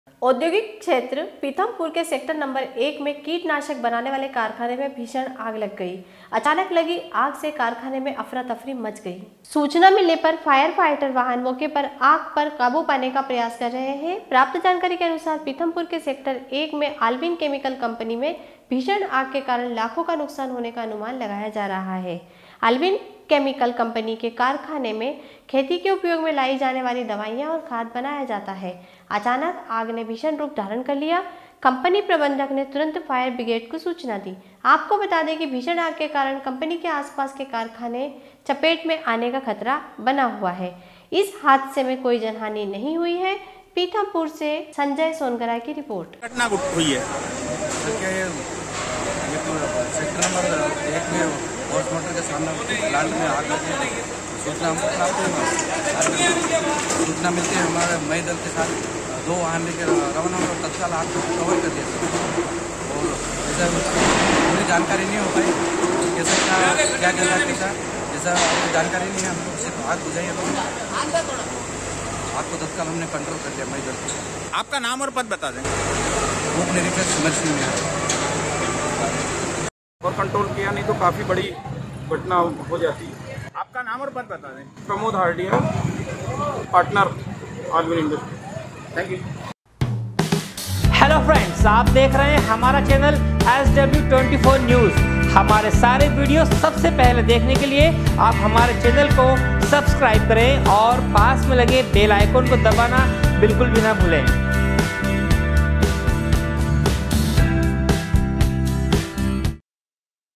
Pithampur- कारखाने में भीषण आग लग गई। अचानक लगी आग से कारखाने में अफरातफरी मच गई
Pithampur- SW 24 News Madhyapradesh
औद्योगिक क्षेत्र पीथमपुर के सेक्टर नंबर 1 में कीटनाशक बनाने वाले कारखाने में भीषण आग लग गई। अचानक लगी आग से कारखाने में अफरातफरी मच गई। पीथमपुर से संजय सोनगरा की रिपोर्ट